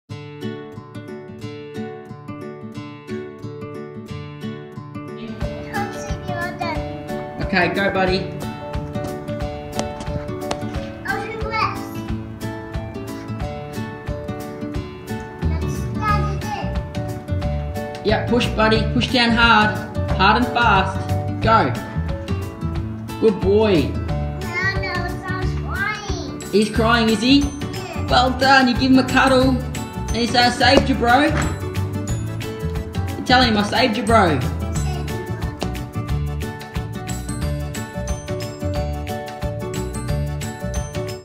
Mr Paramedic teaches his 2 year old CPR
This video is about Mr Paramedic